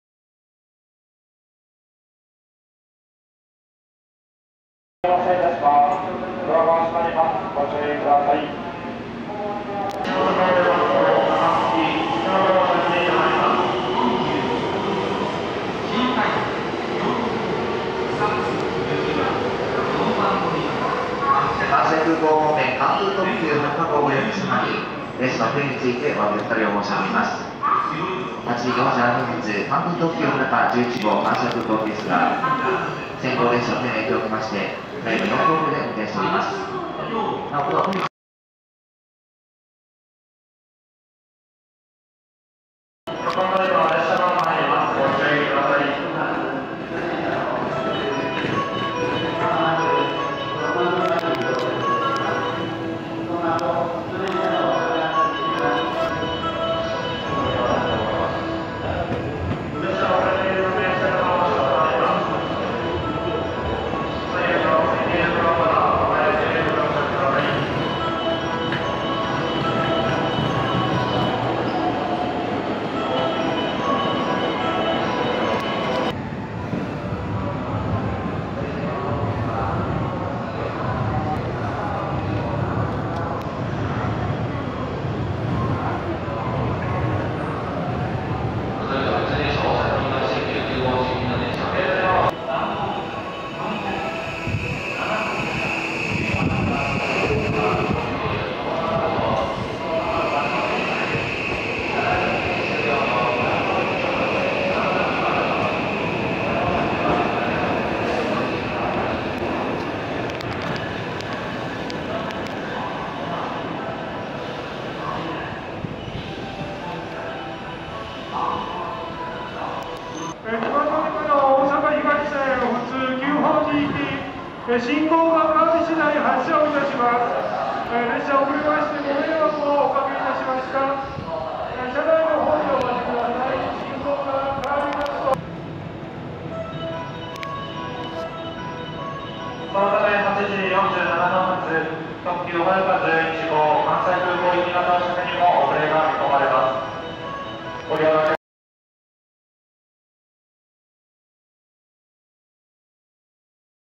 2023/2/13朝の混乱した新大阪駅
梅田貨物線の大阪駅付近新線切り替え後の朝ですが、直通快速用の編成が3本新大阪駅付近にいたり、1番乗り場には試運転の323系が停車しているなどかなりダイヤが乱れていました